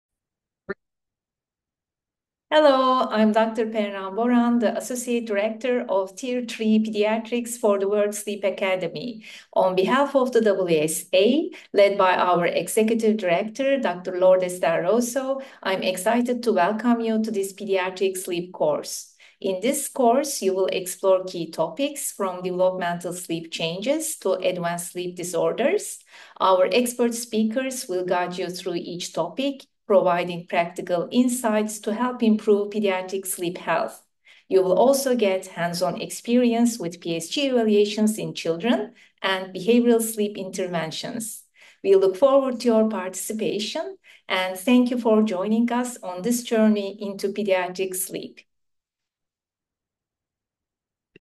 Tier 3 Pediatrics Introduction
Learn more about the Pediatric Track of World Sleep Academy Tier 3 from Dr. Perran Boran!